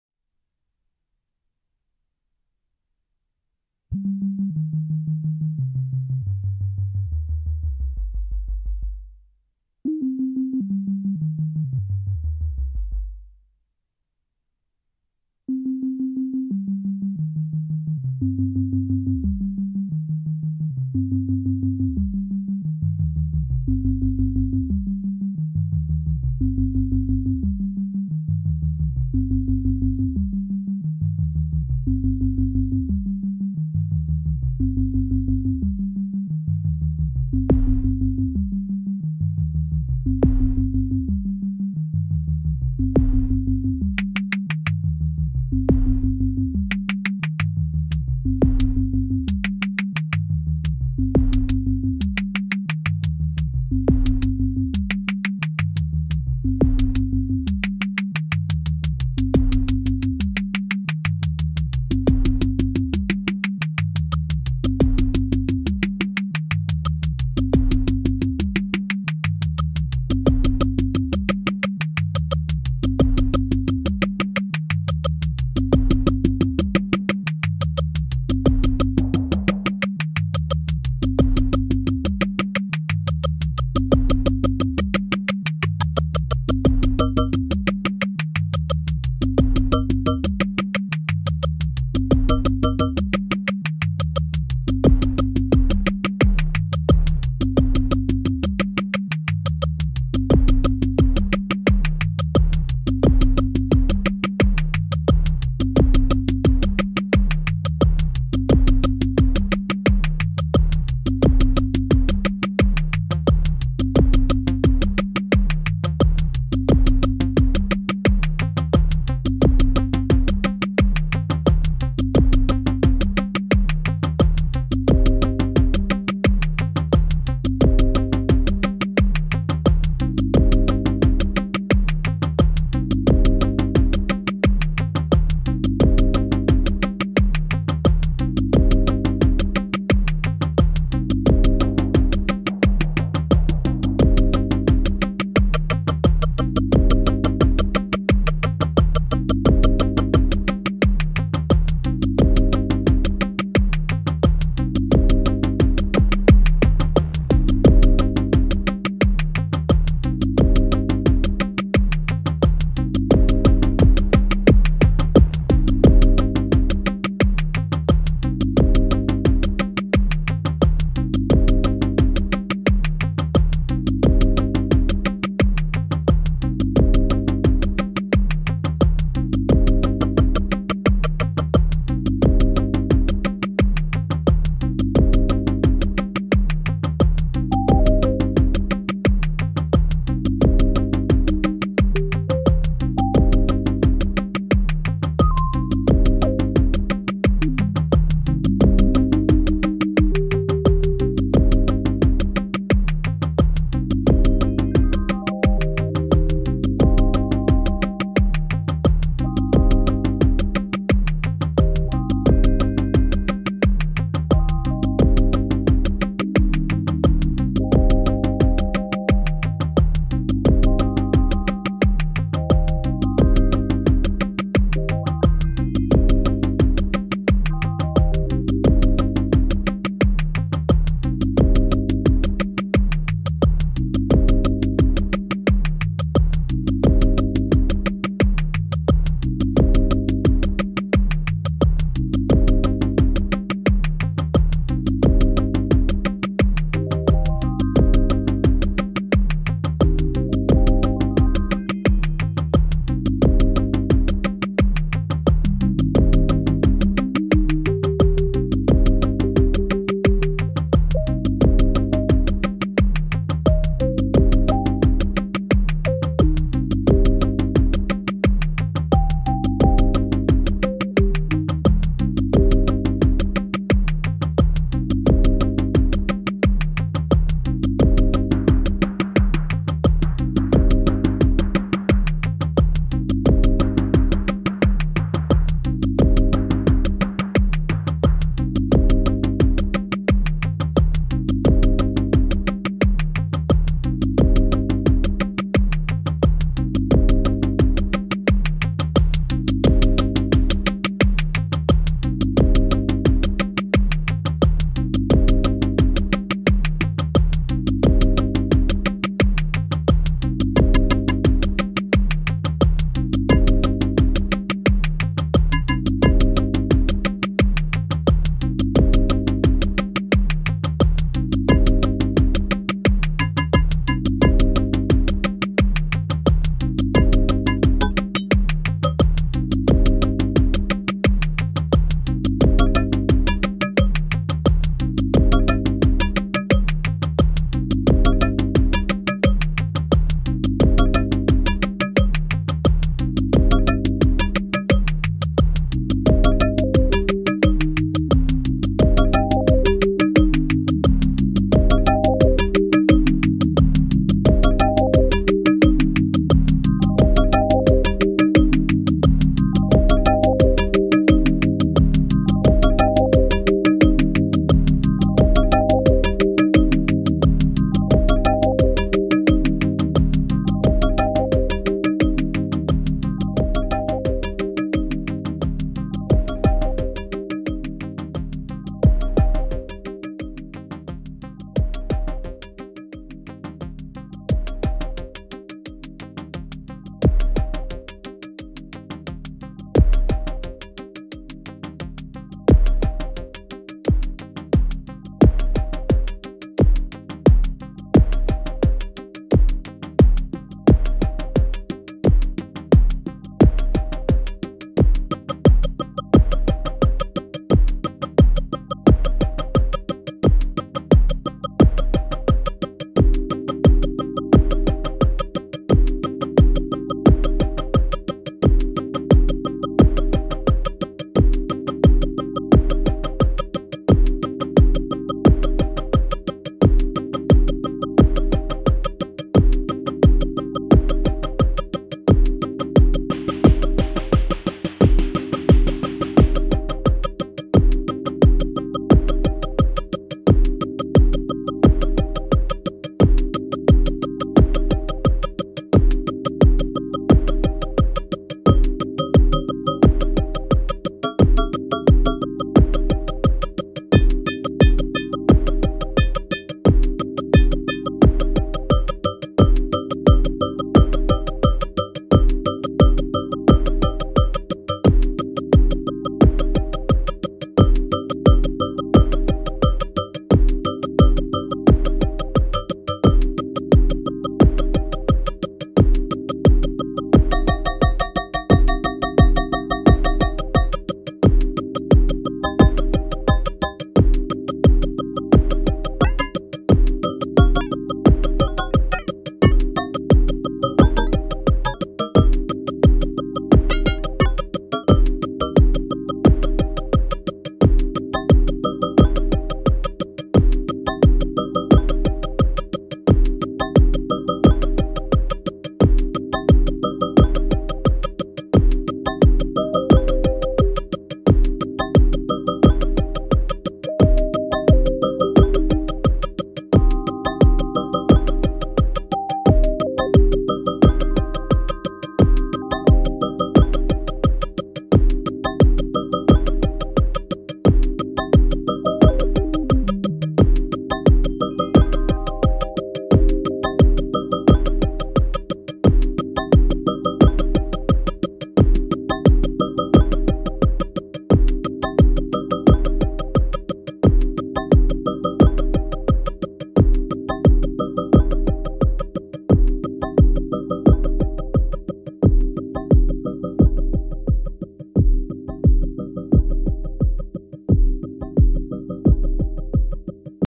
experimental drum looper. axoloti based tigerbox
experimental DIY drum looper based on DIY axoloti tigerbox. using sensel Morph as interface.